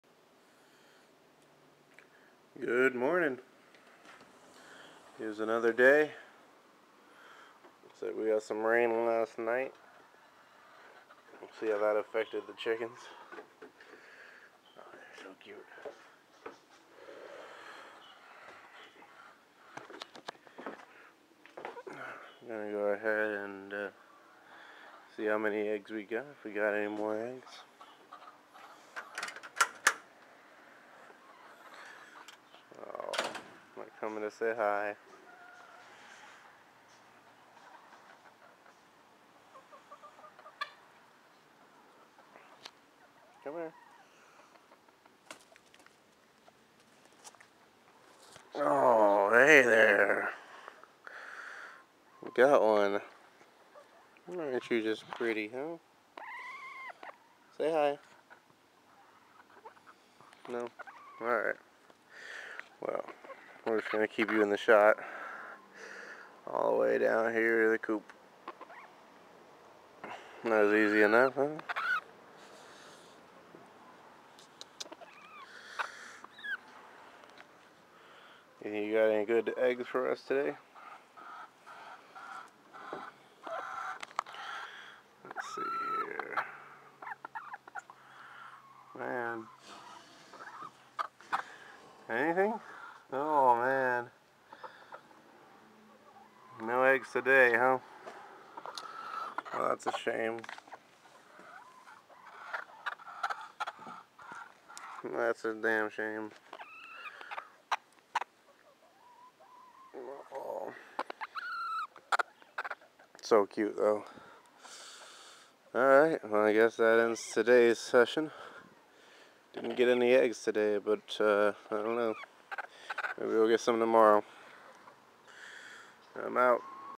Let's get eggs 2
Second day of trying to get eggs from my chickens.